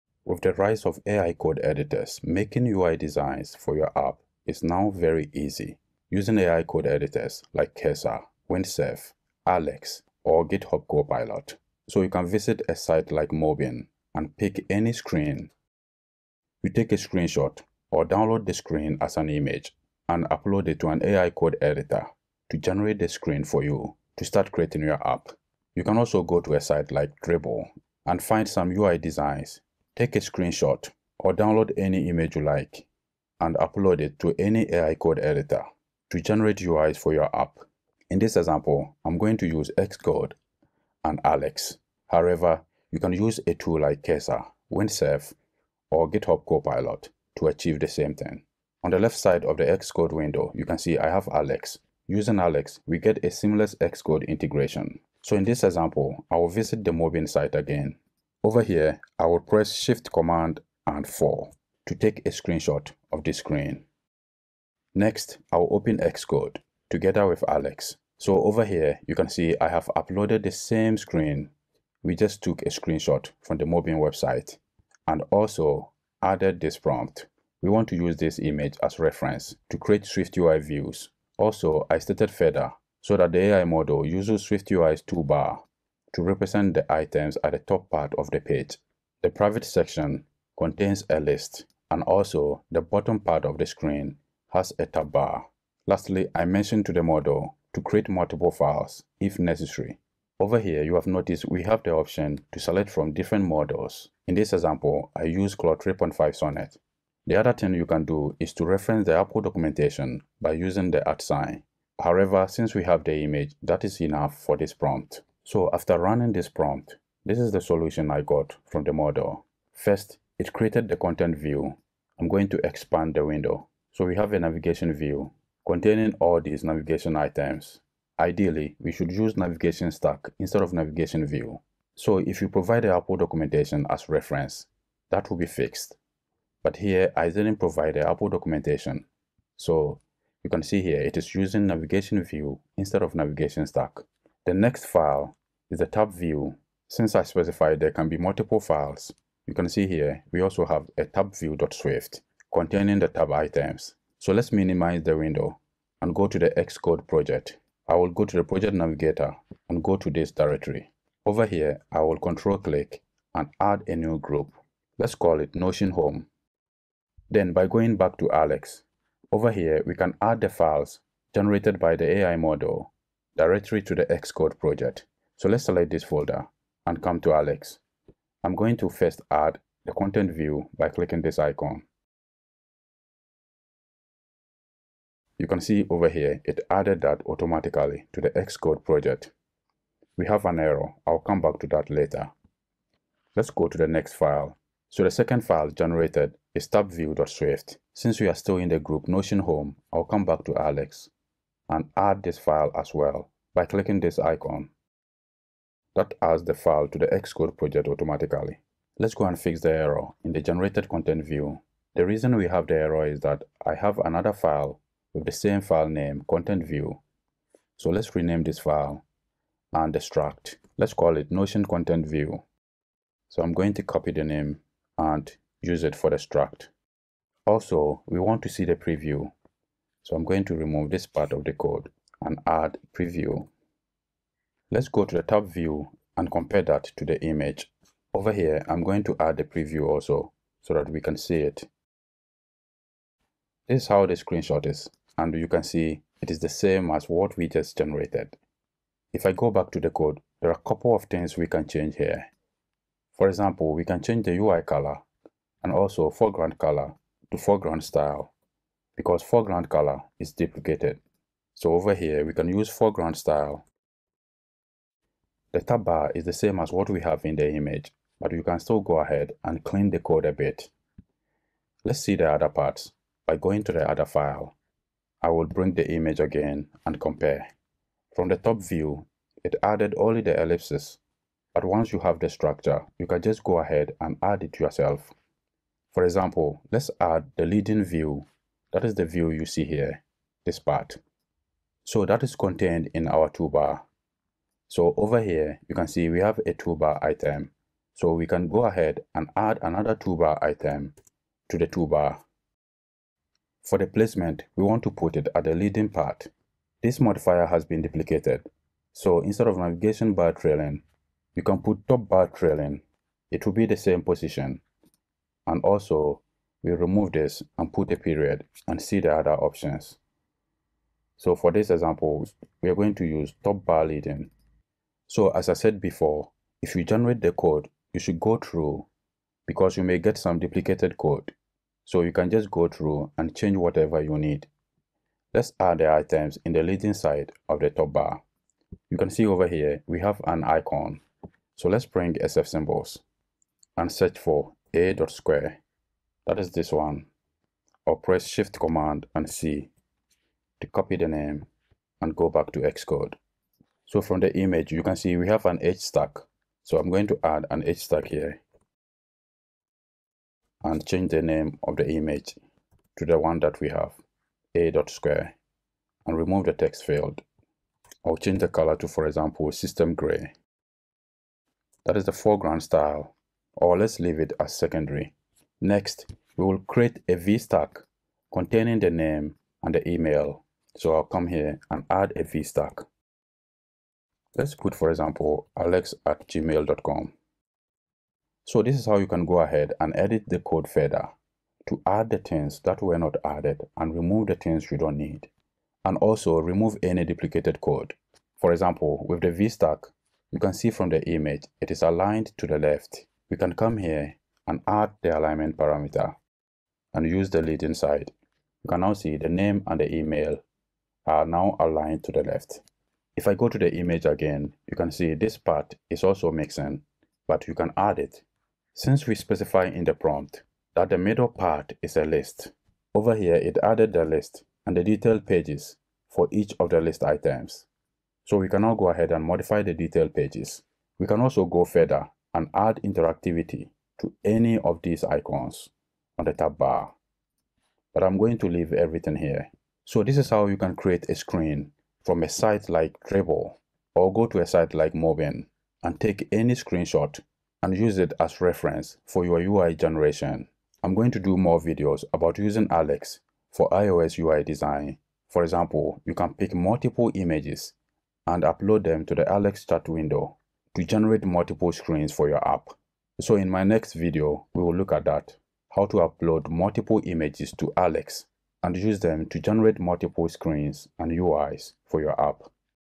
SwiftUI: AI-Assisted iOS UI Design
Let's dive into creating UIs for an iOS app in SwiftUI using an AI Code Editor.